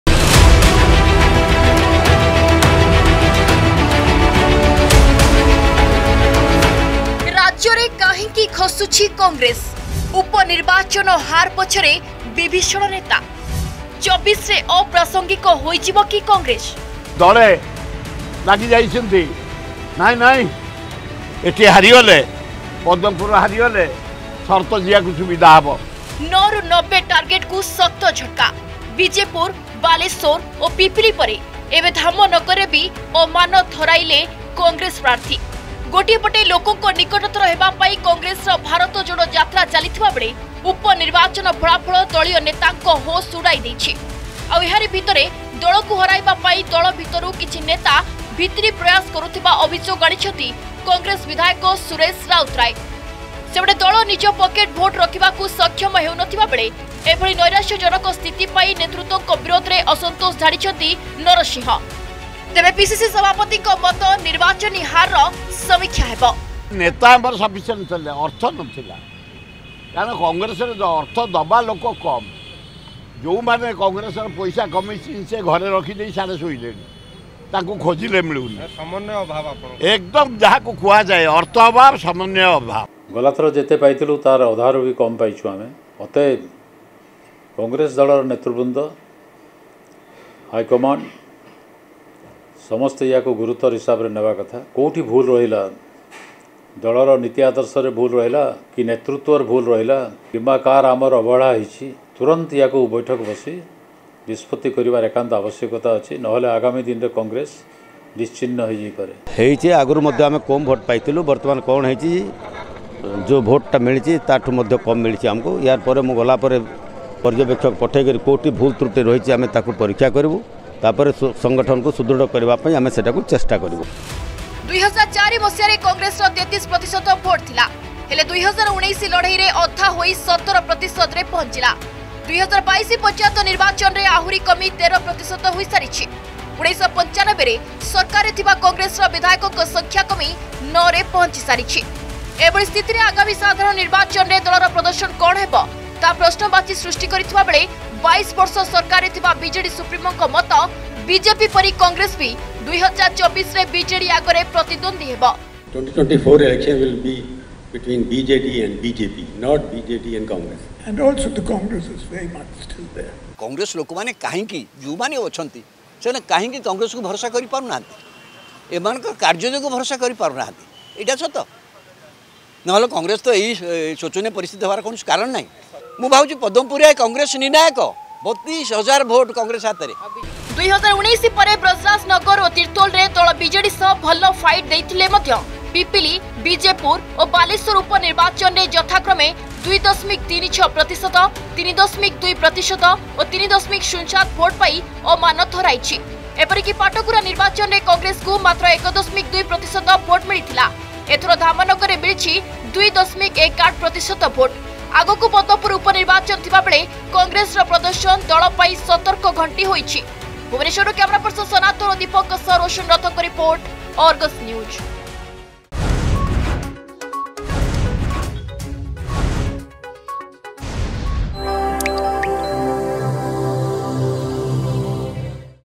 ରାଜ୍ୟରେ କାହିଁକି ଖସୁଛି କଂଗ୍ରେସ ?
୯ ରୁ ୯୦ ଟାର୍ଗେଟକୁ ଶକ୍ତ ଝଟକା। ବିଜେପୁର ,ବାଲେଶ୍ବର ଓ ପିପିଲି ପରେ ଏବେ ଧାମନଗର ରେ ବି ଅମାନତ ହାରାଇଲେ କଂଗ୍ରେସ ପ୍ରାର୍ଥୀ।